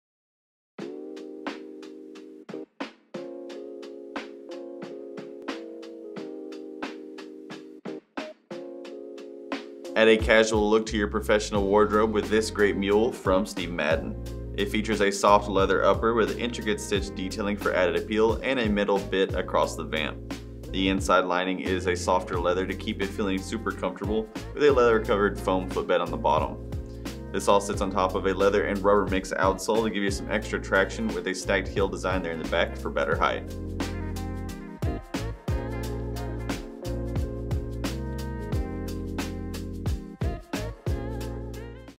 Steve Madden Dazling SKU: 9198146
Day or night, the Steve Madden&reg; Dazling mule delivers distinguished flair.
Genuine leather upper with metal bit detail.
Slip-on with open back.
Round toe with apron stitching.
Man-made lining.
Quilted padded footbed.
Low heel.
Man-made sole.
Imported.